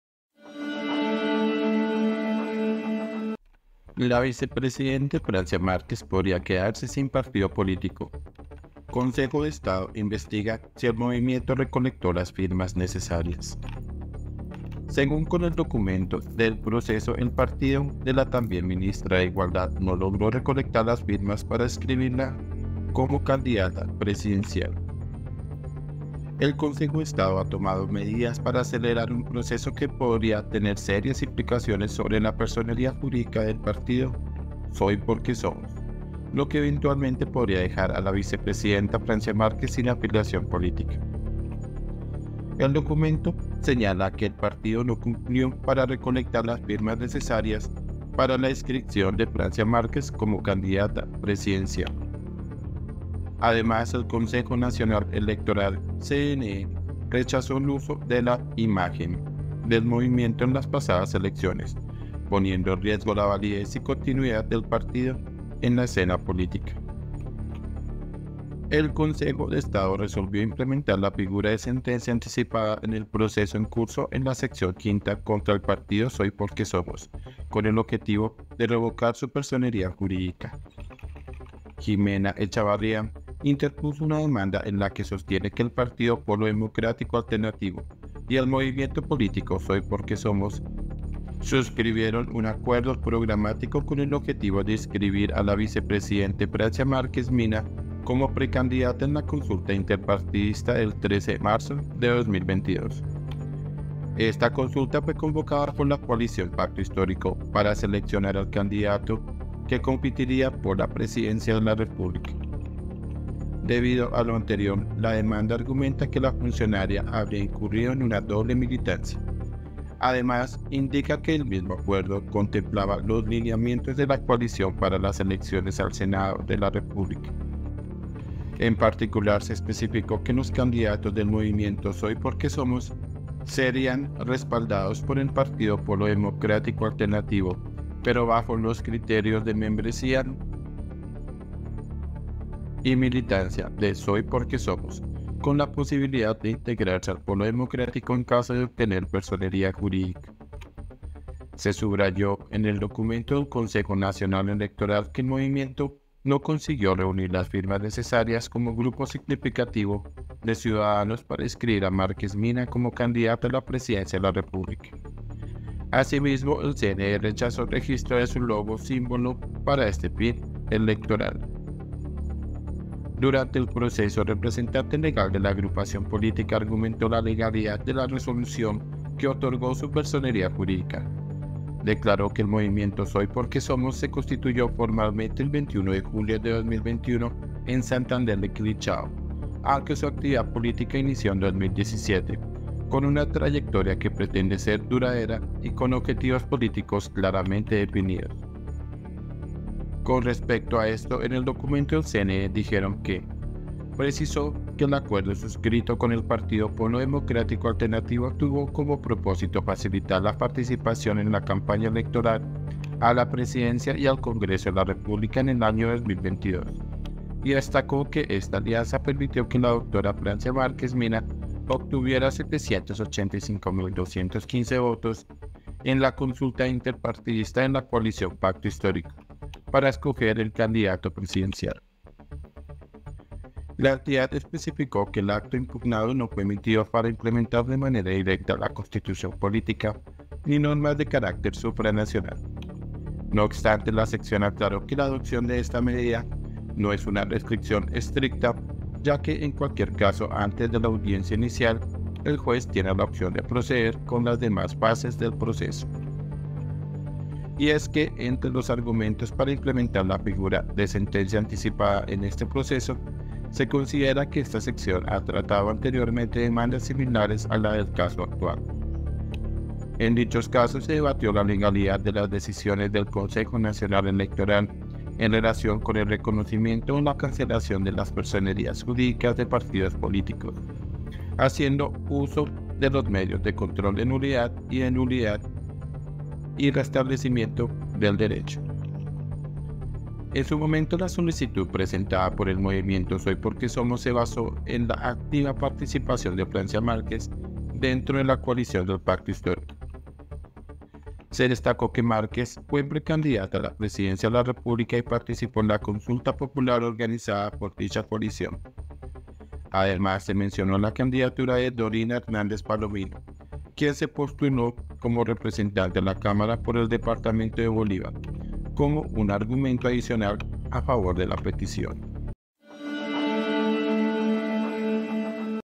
MALAS NOTICIAS PARA FRANCIA MÁRQUEZ 🔴 LA VICEPRESIDENTA PODRÍA QUEDARSE SIN PARTIDO POLÍTICO
La vicepresidenta Francia Márquez podría quedarse sin partido político: Consejo de Estado, investiga si el movimiento recolectó las firmas necesarias
Según con el documento del proceso, el partido de la también ministra de Igualdad no logró recolectar las firmas para inscribirla como candidata presidencial
INFOBAE